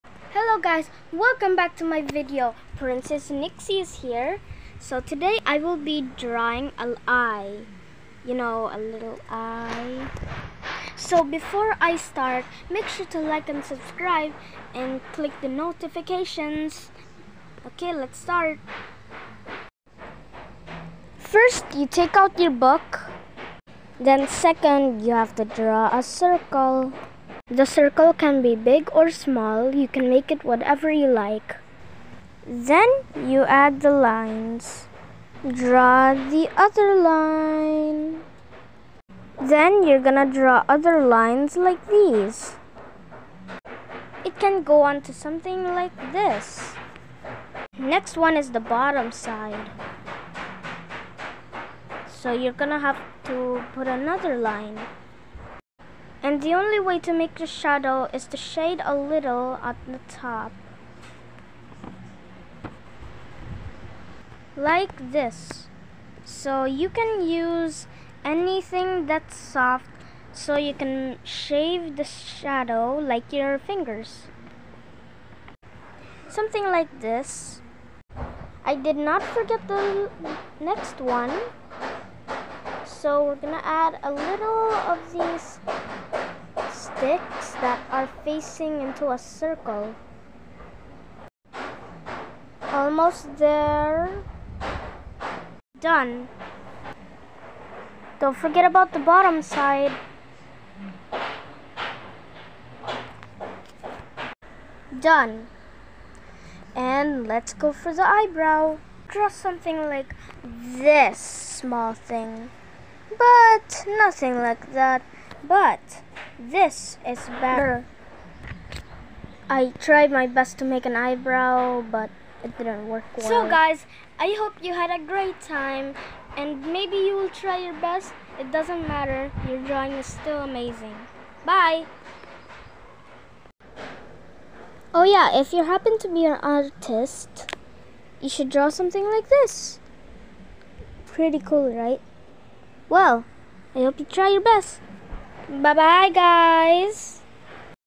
how to draw eyes 👀 // please excuse the hammering / someone's fixing our roof😂
Hey guys,

May I know what do you like to do during your free time?
Me, aside from playing online games, playing with my cousins, I also like to draw.

I hope you will enjoy my drawing. Please excuse the noise and the hammering. Someone's fixing our roof.

 Please come and join me. Have a nice day!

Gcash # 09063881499